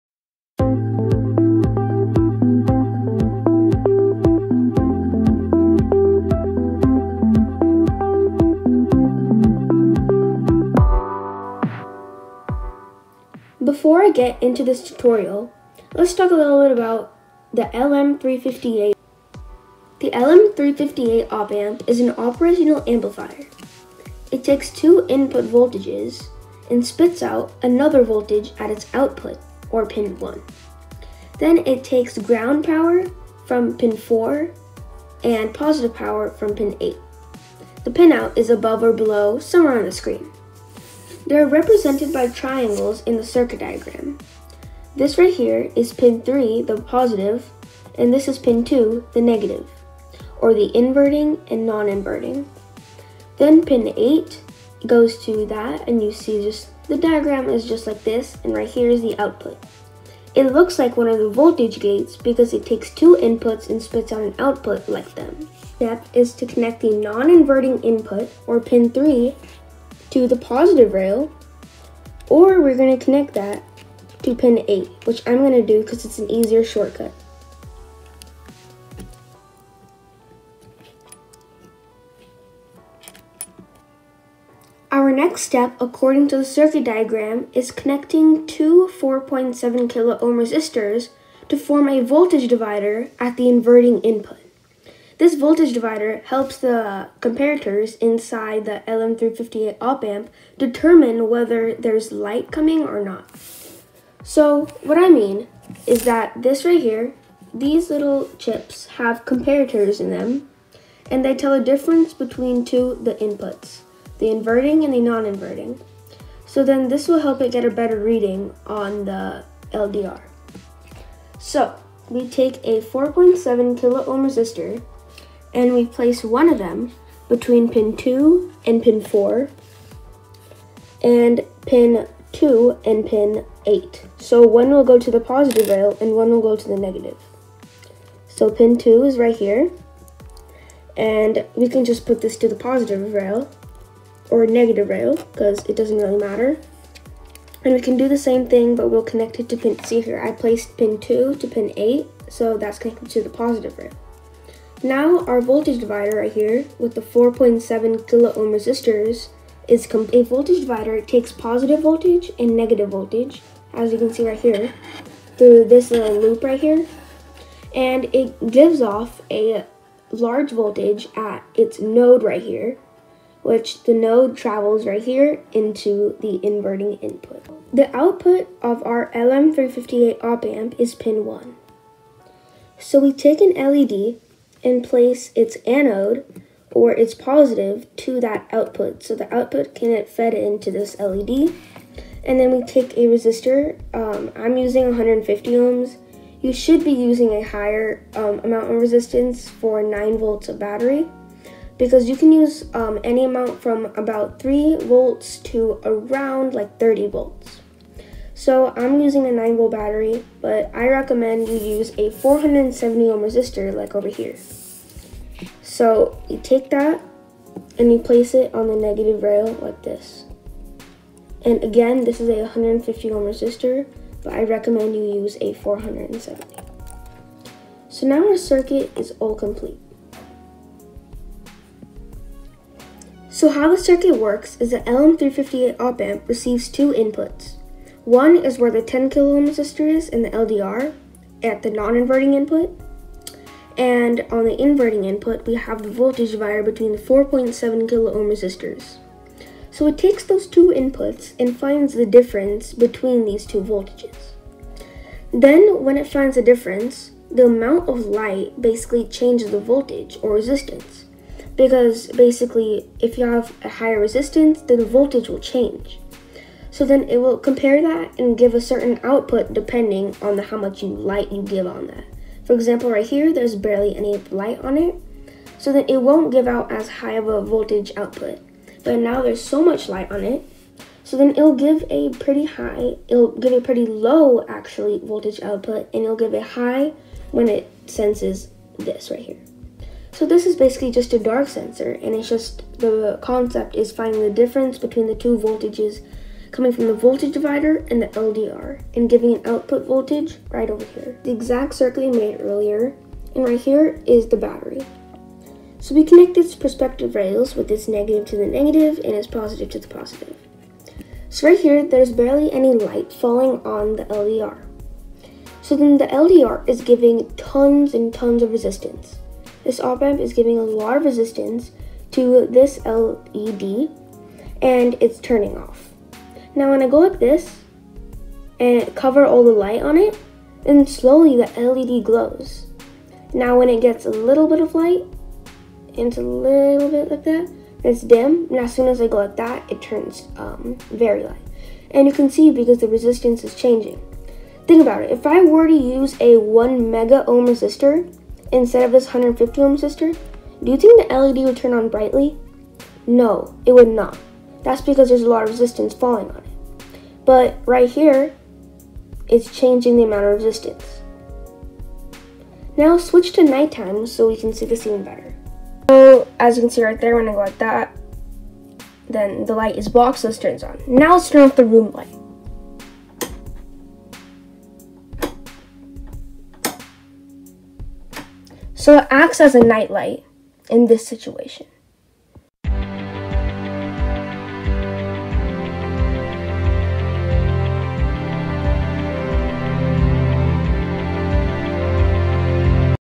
NIGHT LIGHT/Dark Sensor Circuit Using LM358 Op-Amp
Hello Everyone! In this Video, we will make a Night Light/Dark Sensor using the LM358 Op -Amp. An Op-Amp is an IC that receives Inverting and Non-Inverting Inputs and gives off an output. I talk more about it in the video. A Basic explanation of the Theory is the Op amp Receives the LDR Resistance at it's Non-Inverting Input and a Voltage Divider at it's Inverting Input. These two Inputs get compared leading to the LDR to getting light triggering the output to give low voltage.

Components:

-LM358 Op-Amp
-Breadboard
-2x 4.7k Resistors
-10k Resistor
-LED
-Resistor For LED Depending on Supply Voltage

Circuit Diagram: In video as I reference it